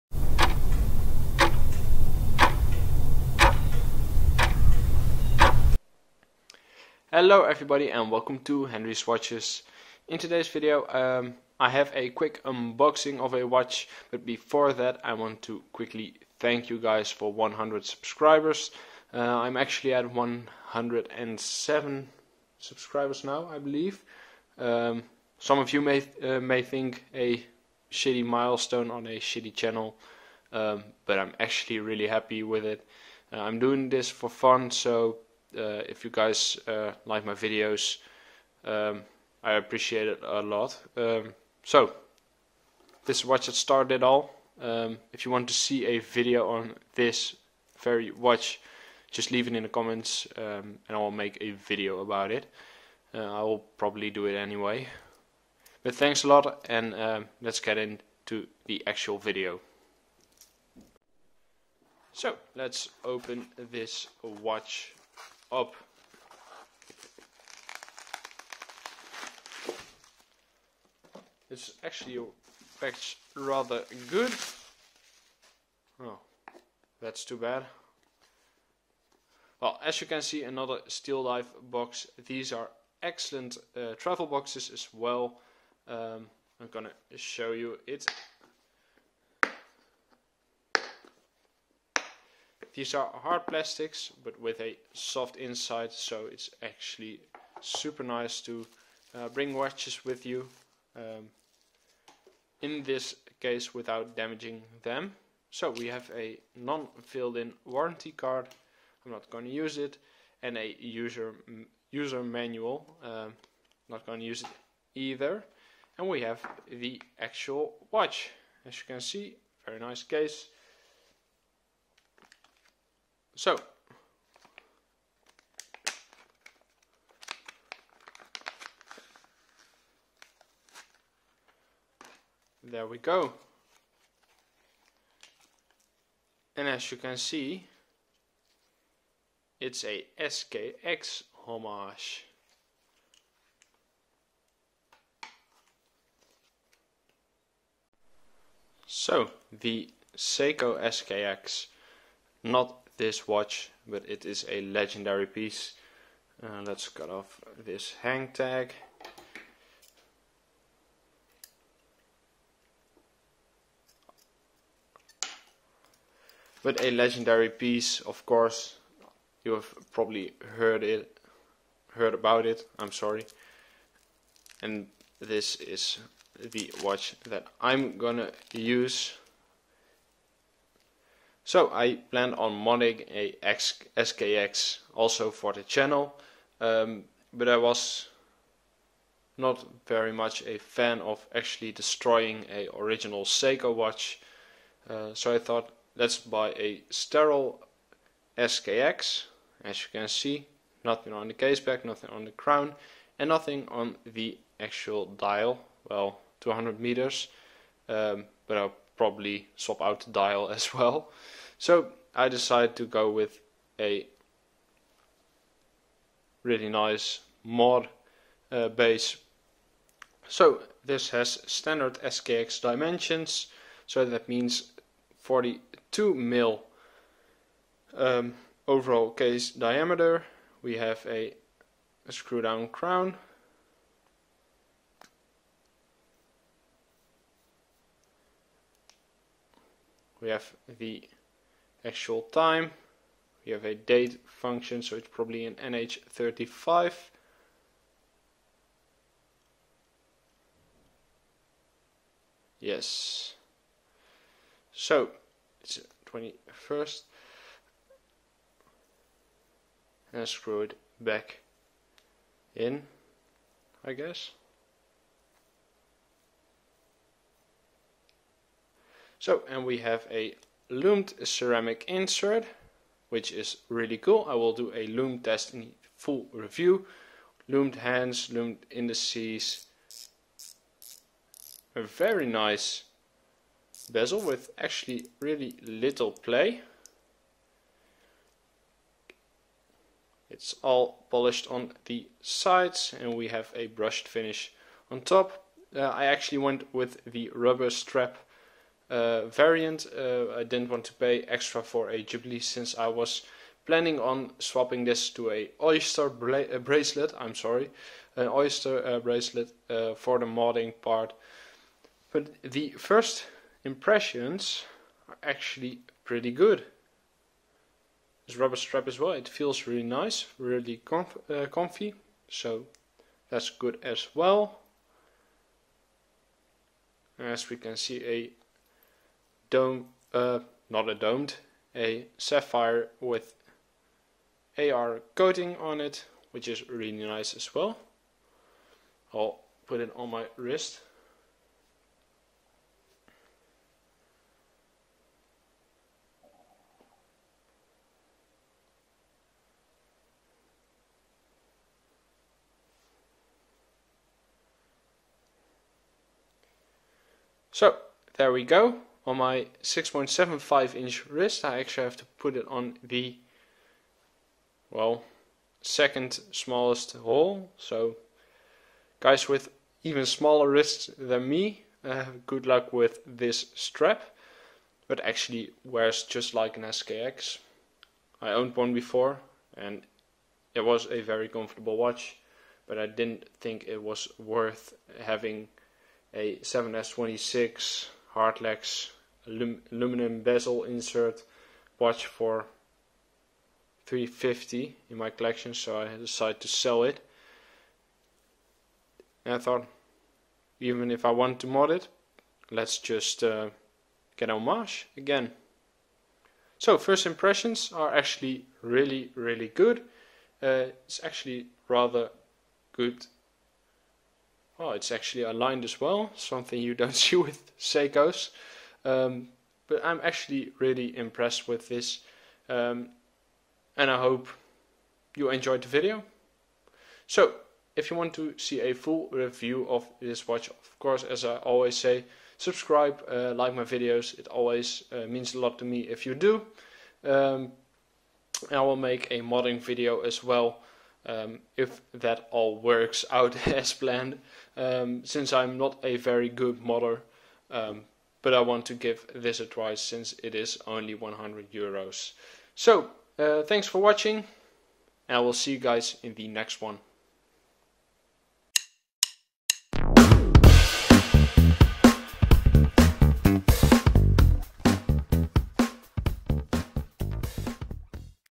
Best Seiko SKX Alternative for Modding? | Steeldive SD1996 Unboxing and First Impressions!!
Hello Everybody,

In this video I unbox and review the Steeldive SKX (SD1996) and give my first impression. I personally think this might be the best mod platform since the the Seiko SKX is getting rare and expensive.

- Henry

STEELDIVE SKX